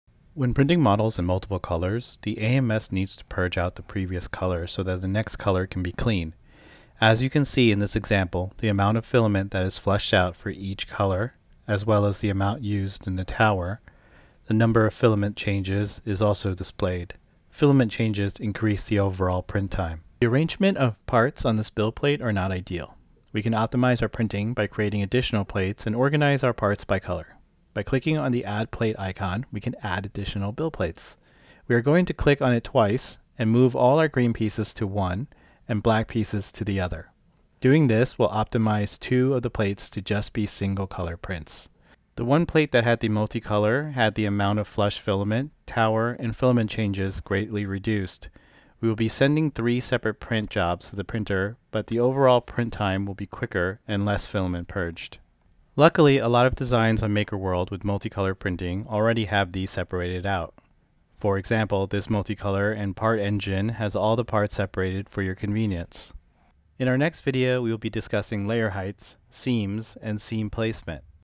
Episode 7: Divide and Print Your Models on Multiple Plates
This video covers how to use multiple plates to print multi-color models.